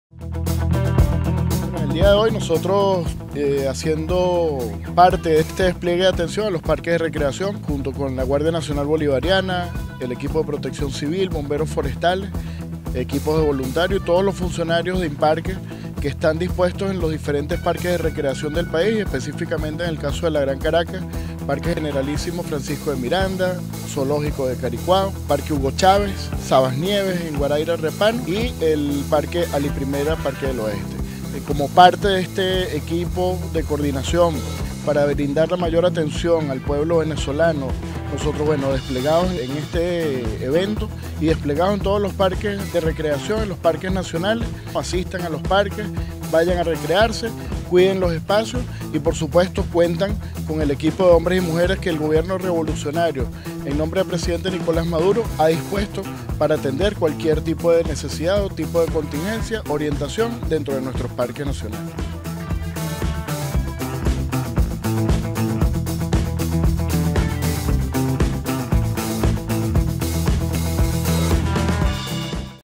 Inparques y PC desplegaron Plan de Atención Integral en parques de la Gran Caracas
En el marco de la Gran Misión Justicia Socialista, el Instituto Nacional de Parques (Inparques), ente adscrito al Ministerio del Poder Popular para Ecosocialismo y Aguas (Minea), en conjunto con Protección Civil (PC) se efectuó este sábado, el despliegue del Plan de Atención Integral (PAI) en el Parque Generalísimo Francisco de Miranda, en la ciudad de Caracas.

En este sentido, los Bomberos Forestales y el Cuerpo Civil de Guardapaques de Inparques, junto a funcionarios de PC se desplegarán en los principales parques de la Gran Caracas las cuales cuentan con mayor afluencia de visitantes como lo son: el  Parque Nacional Waraira Repano, específicamente en el sector Sabas Nieves, el  Parque de Recreación Alí Primera, Generalísimo Francisco de Miranda, Hugo Chávez y el Zoológico de Caricuao